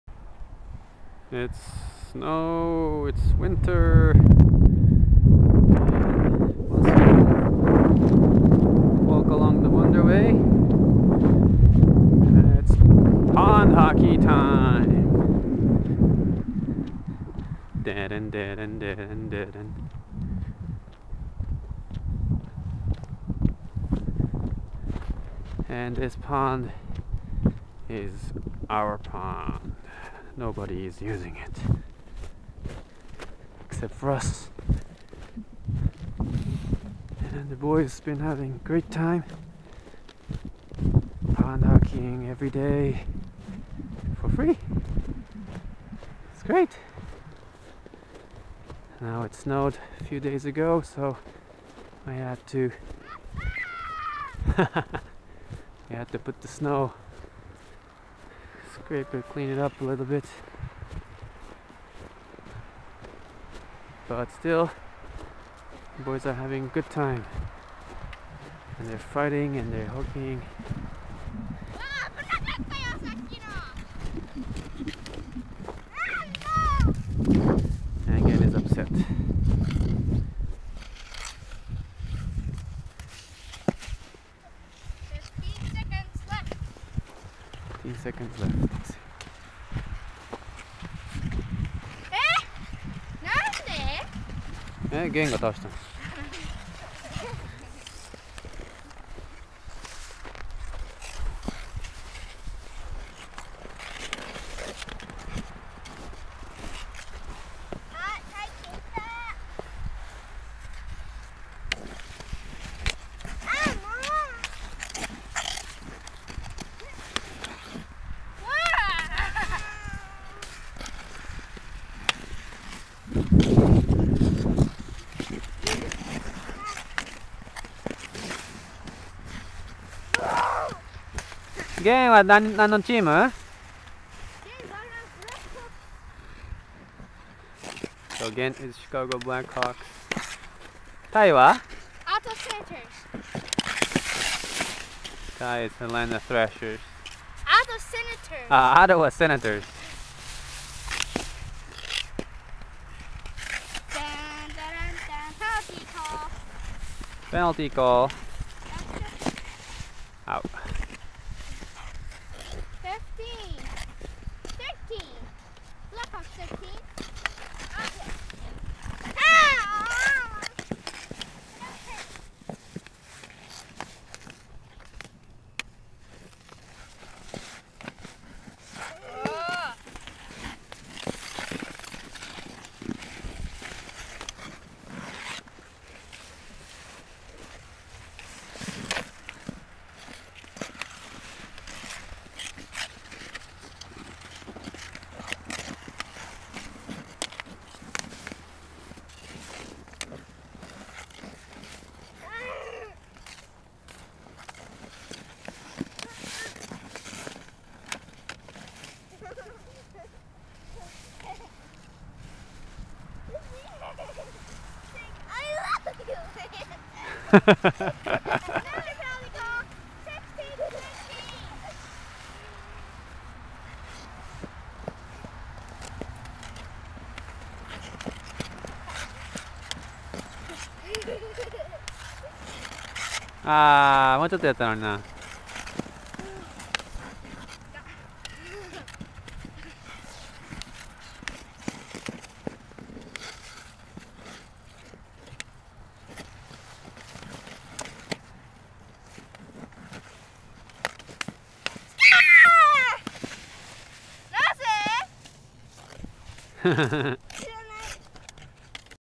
tai vs gen on the pond
今度は泰と玄のみのアイスホッケー